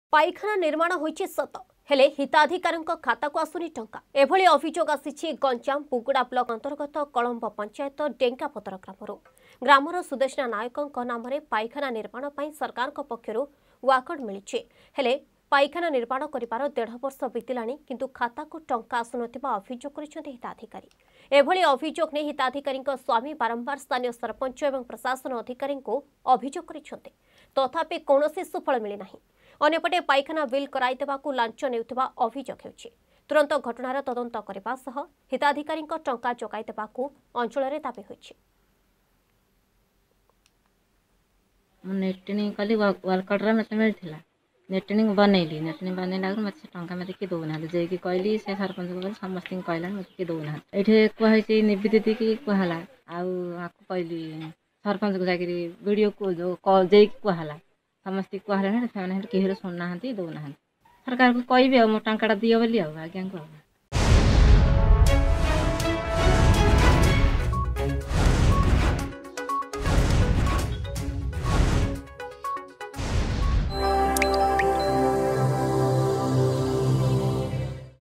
ପାଇଖାନା ନିର୍ମାଣ ହୋଇଛି ସତ ହେଲେ ହିତାଧିକାରୀଙ୍କ ଖାତାକୁ ଆସୁନି ଟଙ୍କା
ଗଞ୍ଜାମ ବୁଗୁଡା ବ୍ଲକ ଅନ୍ତର୍ଗତ କଳମ୍ବ ପଞ୍ଚାୟତ ଡେଙ୍ଗାପଦର ଗ୍ରାମରୁ ଗ୍ରାମର ସୁଦଷ୍ଣା ନାୟକଙ୍କ ନାମରେ ପାଇଖାନା ନିର୍ମାଣ ପାଇଁ ସରକାରଙ୍କ ପକ୍ଷରୁ ୱାର୍କଡ଼ ମିଳିଛି। ପାଇଖାନା ନିର୍ମାଣ କରିବାର ଦେଢ଼ ବର୍ଷ ବିତିଲାଣି। କିନ୍ତୁ ଖାତାକୁ ଟଙ୍କା ଆସୁନଥିବା ଅଭିଯୋଗ କରିଛନ୍ତି ହିତାଧିକାରୀ.